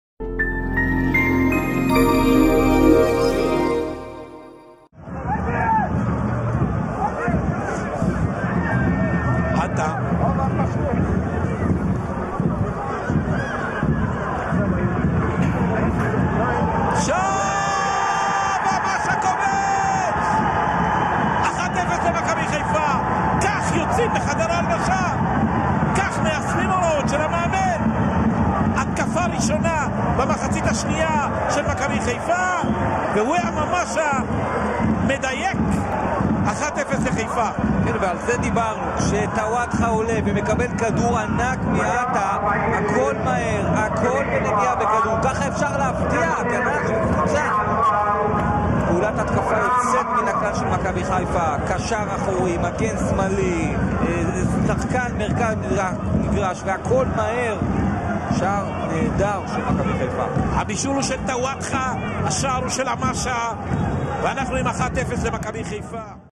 בני סכנין - מכבי חיפה 1:0, עמאשה כובש!
Bnei sahnin - Maccabi Haifa 0:1- Amasha scores!
ליגת העל עונת 2012/13 מחזור 23
דקה 47, 0:1 לחיפה: התקפה נהדרת של הירוקים התחילה בעטאא ששלח כדור ארוך מהמרכז באלכסון שמאלה, טוואטחה המהיר הגיע אליו ובנגיעה העביר למרכז כדור נפלא אליו הגיע עמאשה שבנגיעה מצא את הרשת, שער נהדר.